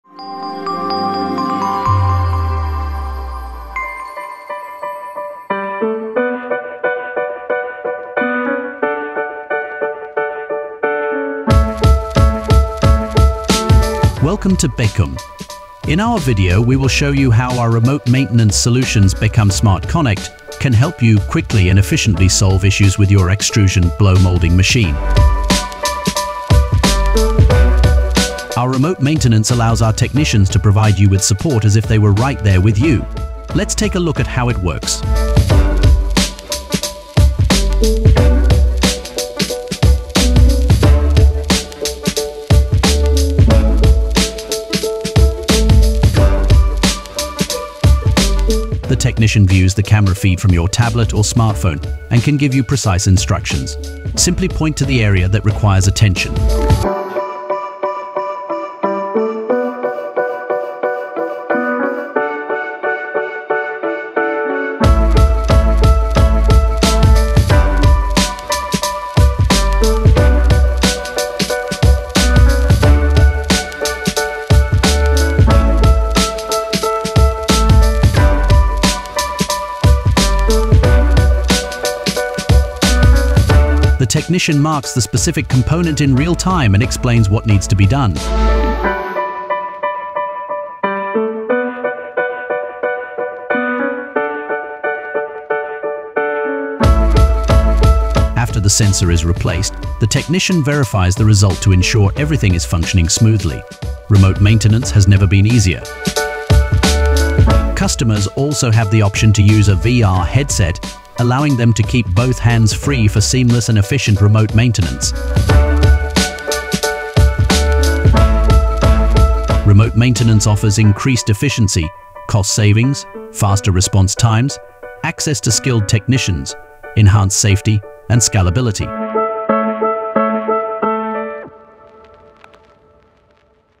Bekum SmartConnect: Efficient Remote Support for Blow Moulding Machines
Bekum SmartConnect offers streamlined, cost-effective remote support for blow moulding machines, reducing the need for on-site technician visits. Connect instantly with our experts for real-time guidance and troubleshooting, saving both time and travel expenses.

Key benefits of Bekum SmartConnect:

• Cost Savings: Eliminate technician travel expenses with remote assistance.
• Immediate Support: Get quick, expert solutions to minimize downtime.
• Hands-Free Convenience: Use an AR headset to keep both hands free while following live instructions.
• Increased Efficiency: Real-time guidance ensures fast, effective troubleshooting.

Bekum SmartConnect is an innovative remote maintenance solution that keeps your operations running smoothly while reducing costs.